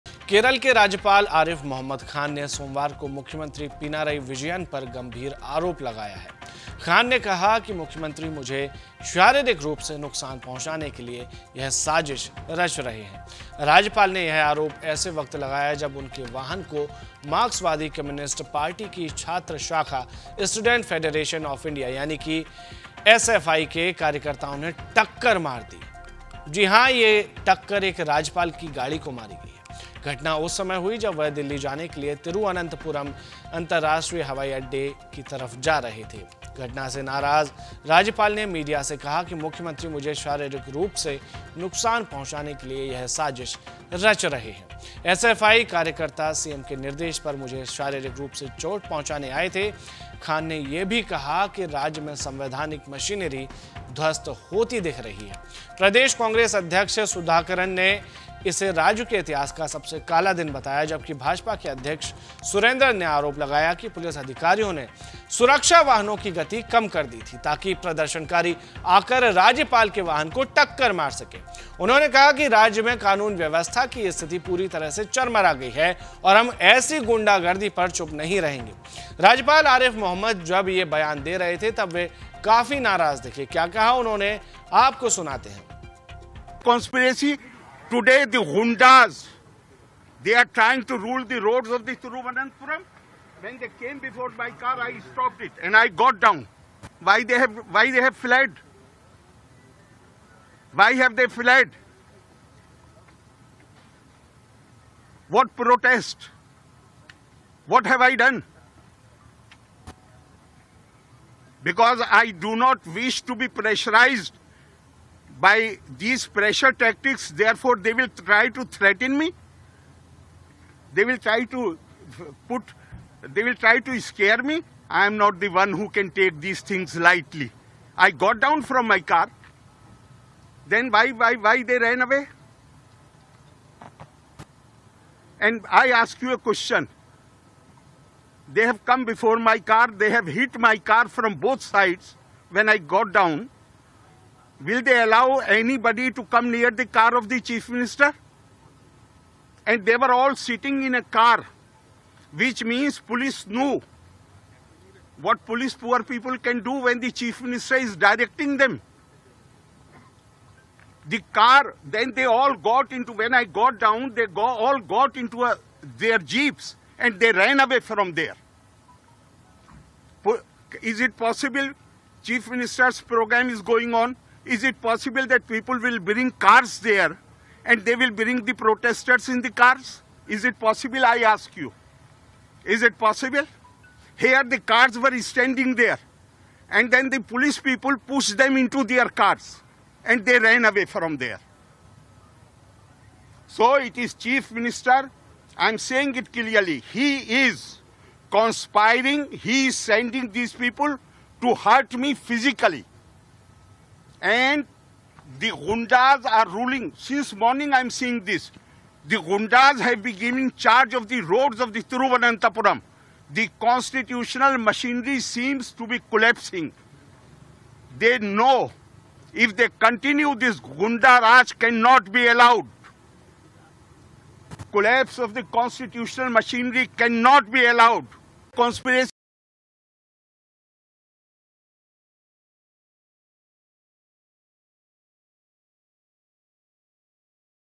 Kerala के Governor Arif Mohammed Khan पर हमला Pinarayi Vijayan पर लगाया आरोप |Kerala Governor attack
केरल के राज्यपाल आरिफ मोहम्मद खान ने सोमवार को मुख्यमंत्री पिनराई विजयन पर गंभीर आरोप लगाया है। खान ने कहा कि मुख्यमंत्री मुझे शारीरिक रूप से नुकसान पहुंचाने के लिए यह साजिश रच रहे हैं। राज्यपाल ने यह आरोप ऐसे वक्त लगाया जब उनके वाहन को मार्क्सवादी कम्युनिस्ट पार्टी की छात्र शाखा स्टूडेंट्स फेडरेशन ऑफ इंडिया (एसएफआइ) के कार्यकर्ताओं ने टक्कर मार दी। घटना उस समय हुई जब वह दिल्ली जाने के लिए तिरुअनंतपुरम अंतरराष्ट्रीय हवाई अड्डे जा रहे थे। घटना से नाराज राज्यपाल ने मीडिया से कहा कि मुख्यमंत्री मुझे शारीरिक रूप से नुकसान पहुंचाने के लिए यह साजिश रच रहे हैं।